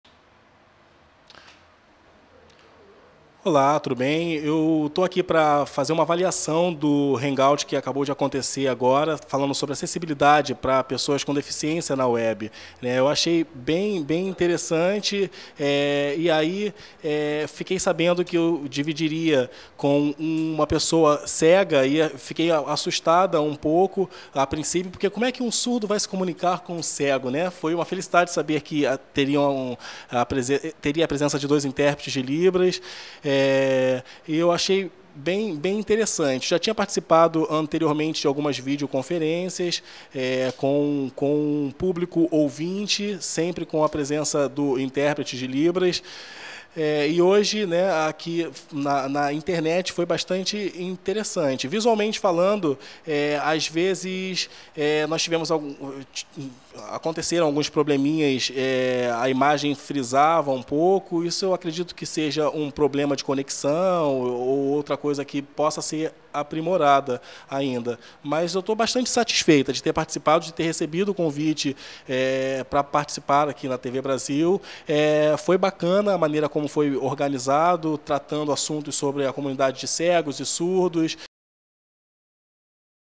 Ana Regina Campelo (Feneis) avalia debate entre surda e cego pela internet
Ana Regina Campelo, presidenta da Federação Nacional de Educação e Integração dos Surdos (Feneis), diz que inicialmente se assustou com a ideia de participar de um debate ao vivo pela internet com um cego, mas revela que a presença de intérpretes da Língua Brasileira de Sinais (LIBRAS) permitiu que o hangout promovido pela EBC (Empresa Brasil de Comunicação) fosse um sucesso. Ana Regina afirma que, em alguns momentos, enfrentou problemas para entender a tradução por conta da qualidade da internet, mas acredita que iniciativas como esta devem continuar.

Ana Regina Campelo participou de um debate sobre acessibilidade na web para o portal da EBC com Marco Antônio Queiroz, que é cego, autor do site Bengala Legal e membro da equipe que desenvolveu o Modelo de Acessibilidade em Governo Eletrônico (e-mag).

A mediação foi da jornalista Carla Maia, da TV Brasil. Os intérpretes de LIBRAS foram: Mariza Castro (EBC Serviços), Jhonatas Narciso (Jornal Visual/TV Brasil) e Claudia Jacob (Jornal Visual/TV Brasil).